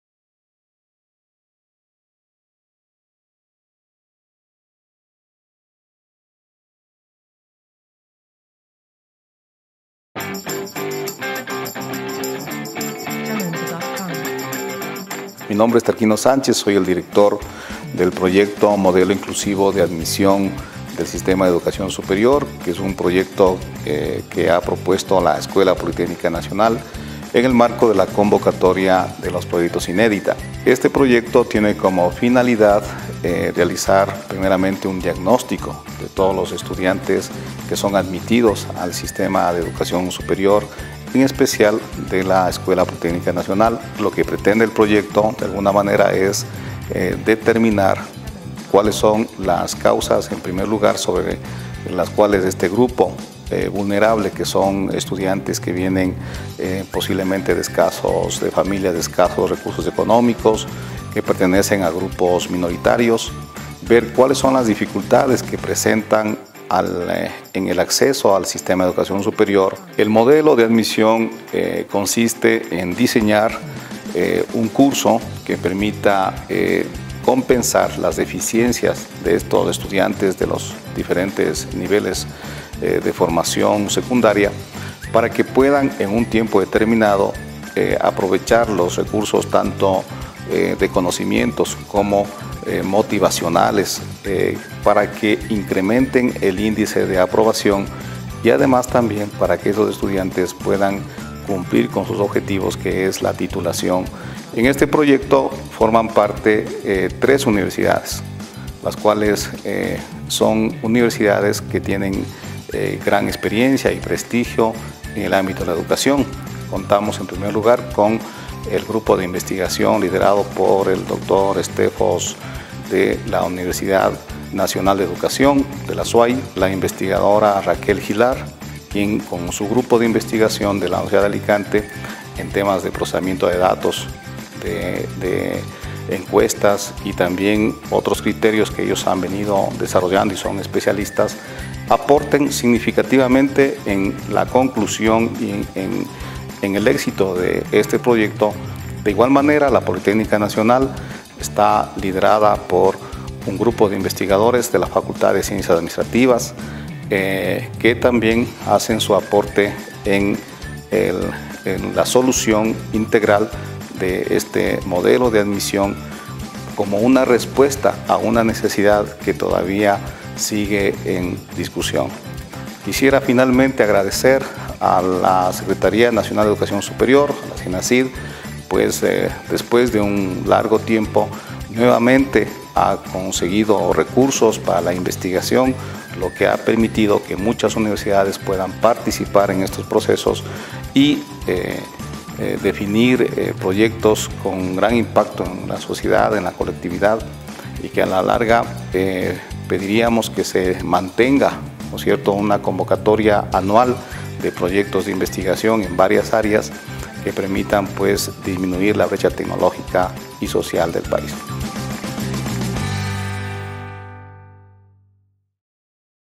Ganador Convocatoria Inédita - Cápsula 2
Diseño e Implementación de un Modelo Inclusivo de Admisión
para el Sistema de Educación Superior en el Ecuador.